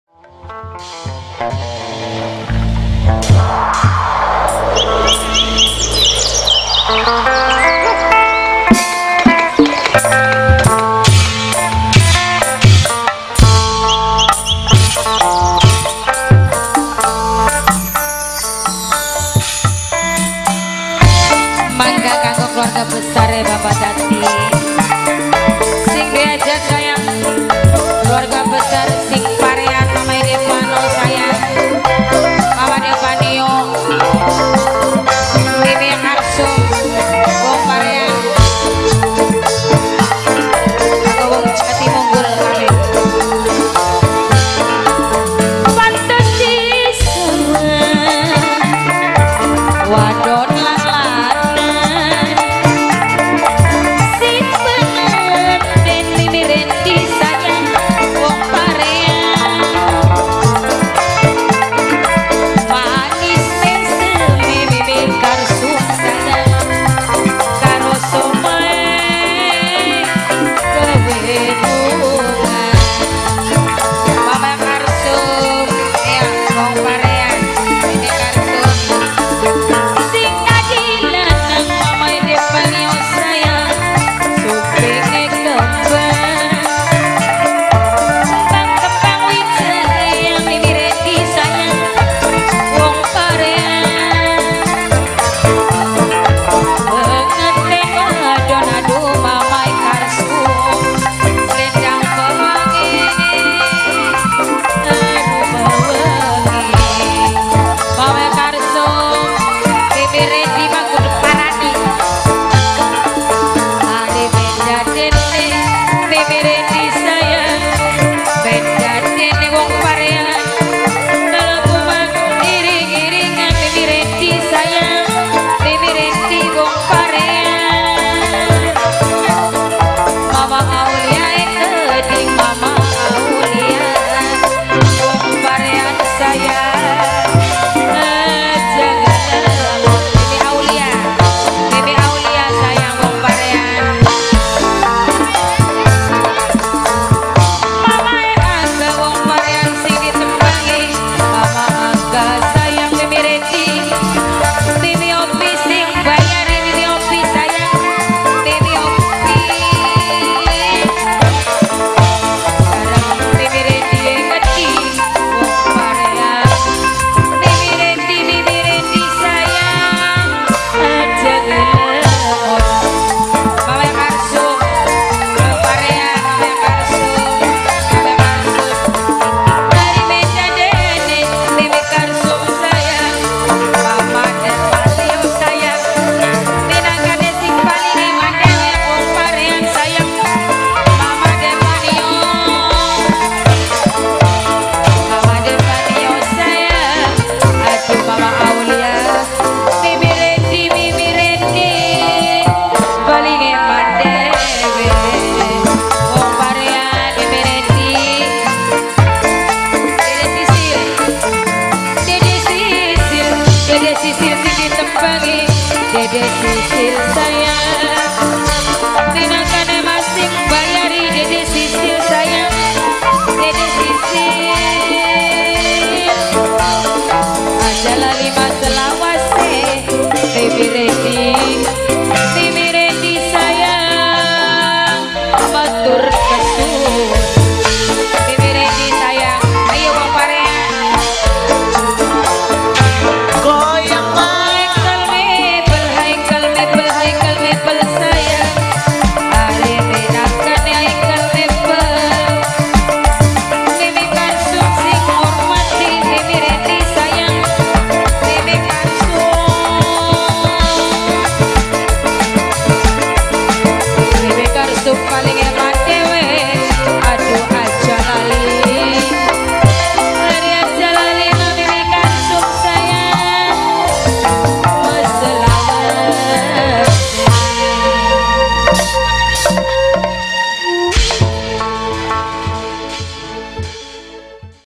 TEMOAN - VOC.MIMI INIH - PUTRA SURTI MUDA - 20 JUNI 2019 - RANCAHAN ( ARYA PRODUCTION )
Pelaksanaan : Rabu 03 Juli 2019
Pesta Hajatnya : Kel. Bpk. Datim / Ibu.Eliyah
Tempat : Desa Jatimunggul Blok Buyut Jati Rt.02/02
Kec. Trisi - Indramayu